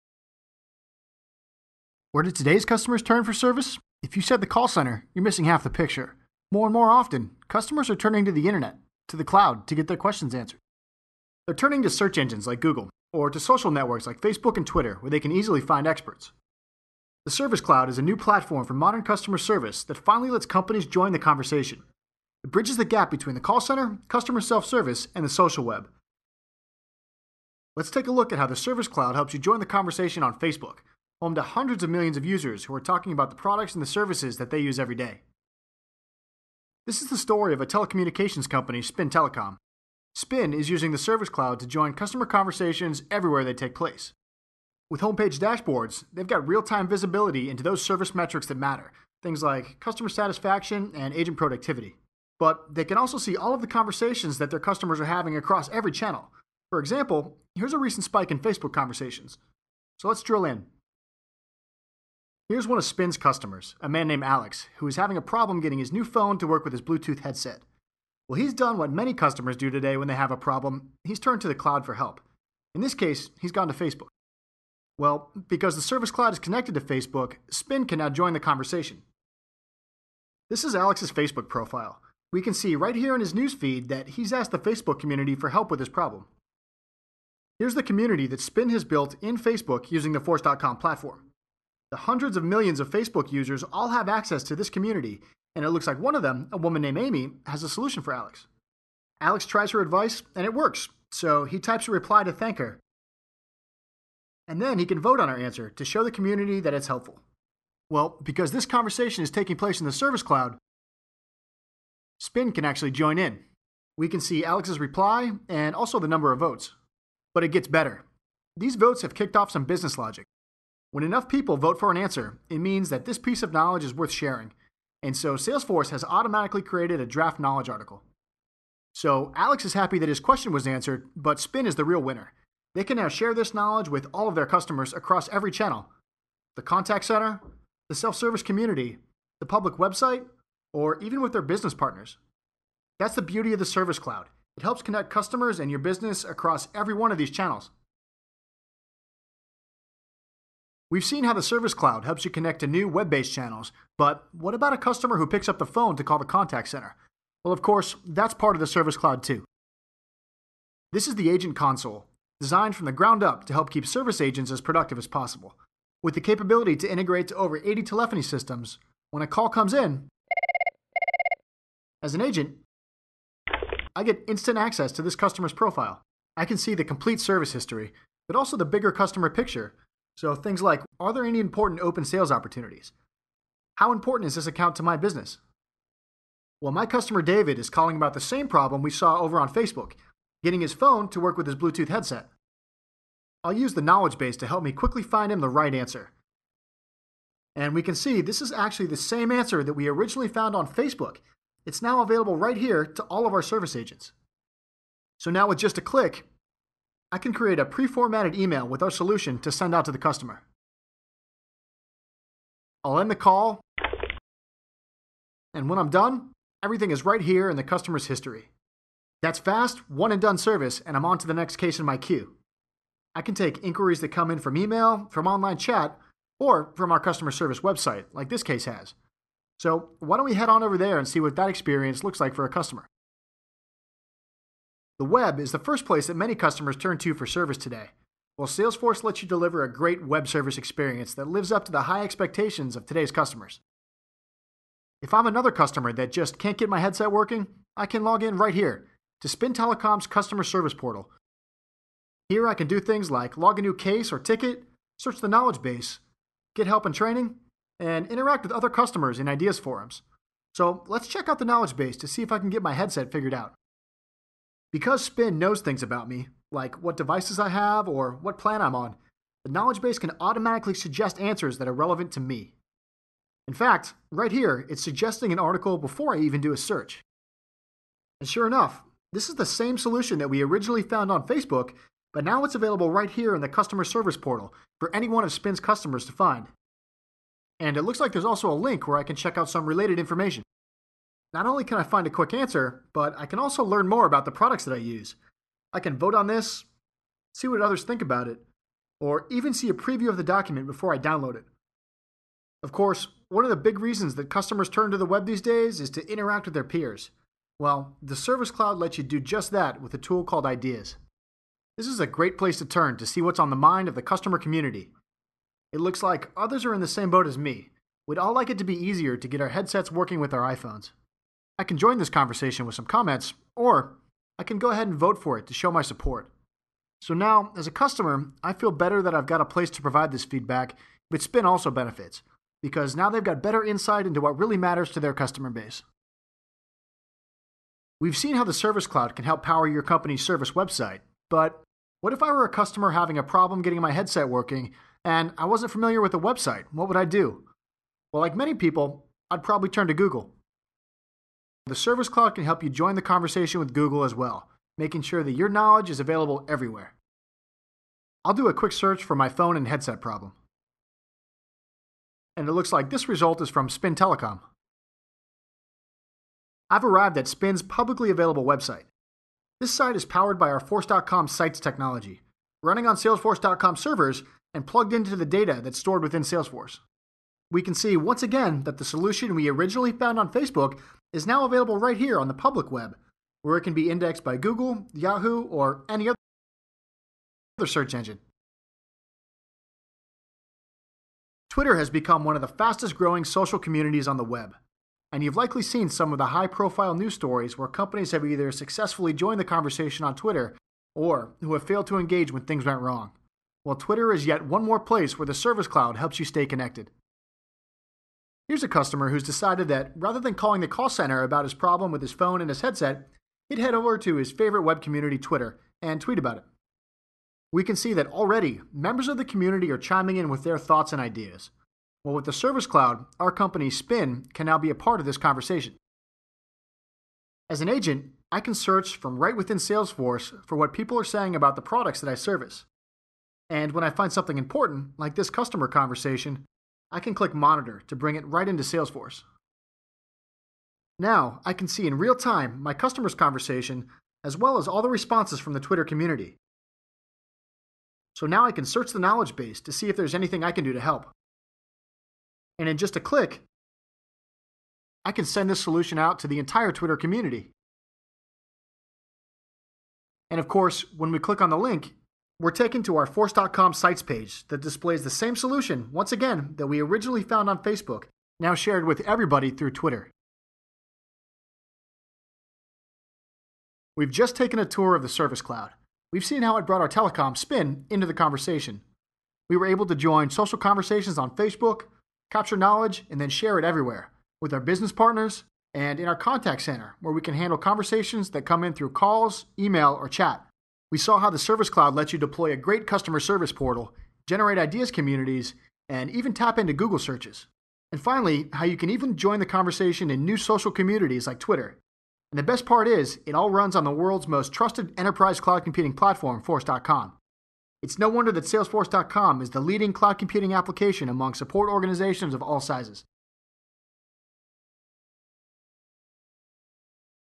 ARCHIVED: Salesforce.com: Service Cloud Demo
Brief demo of the Salesforce.com Sales Cloud product